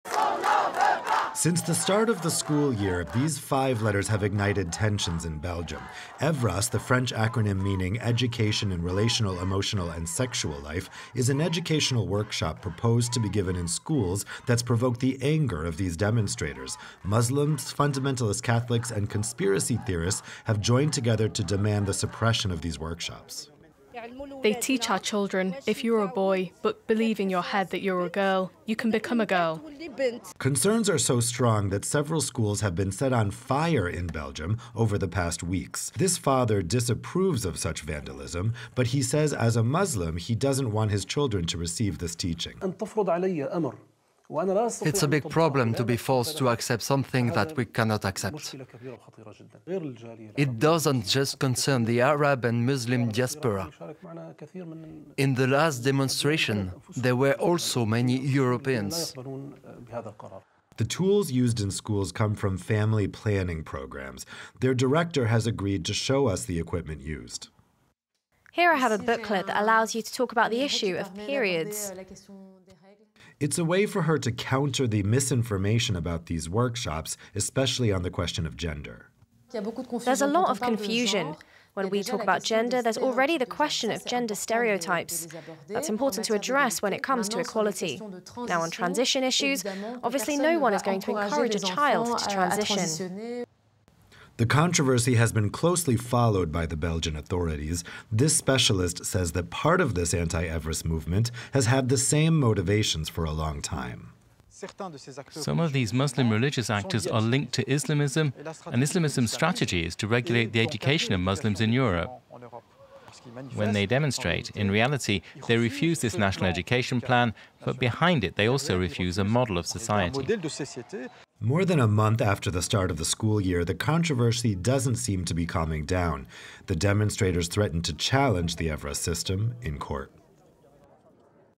‘A satanic guide’: Sex-ed classes in Belgian schools unites Catholics and Muslims in protest
The start of the school year in Belgium has been turbulent, and even violent, amid a controversy that has not calmed down four weeks after classes resumed. The controversy surrounds a workshop on sex education called “EVRAS”. Religious extremist circles – both Catholics and Islamists - have been whipping up opposition. Several demonstrations have taken place denouncing “a satanic guide useful to pedophiles”, as one protester put it, and eight schools were burned down across the country.